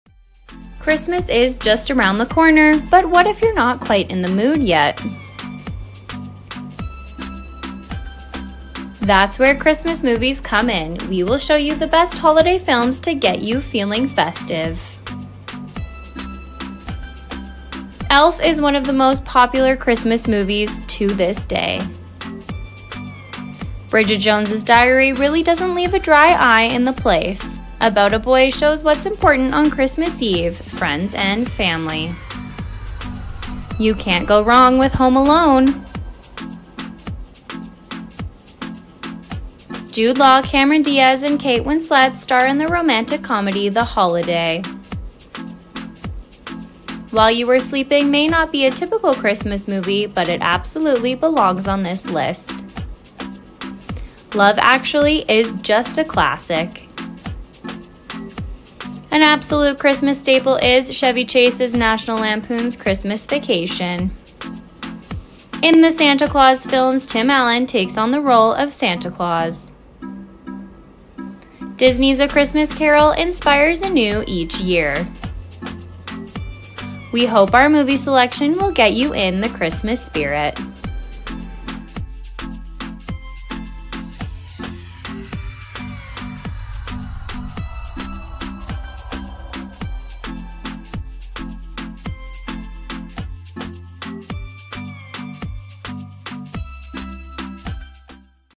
From 'Elf' to 'Home Alone': Movies To Get You Into The Christmas Spirit! | Celebrity Hot Goss
In the time leading up to Christmas, the best way to get in the mood for the holiday season is to watch a movie. We show you the 10 best films to get you into the Christmas spirit!